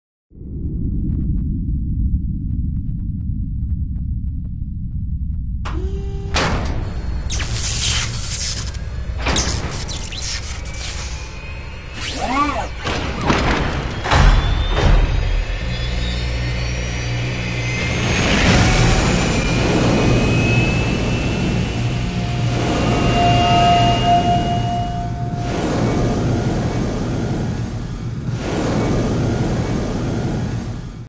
WeltallCinematicFinal
A fan made cinematic of Weltall transforming to Weltall-Id. Weltall/Weltall-Id is the gear piloted by the main character in Xenogears, a japanese role playing game made by Squaresoft (now known as SquareEnix).

This video is real-time in the Unreal Development Kit. All artwork was done by me, sound work was done by Luke Jayapalan. Weltall, Weltall Id, and Xenogears are copyright of Squaresoft/Square Enix.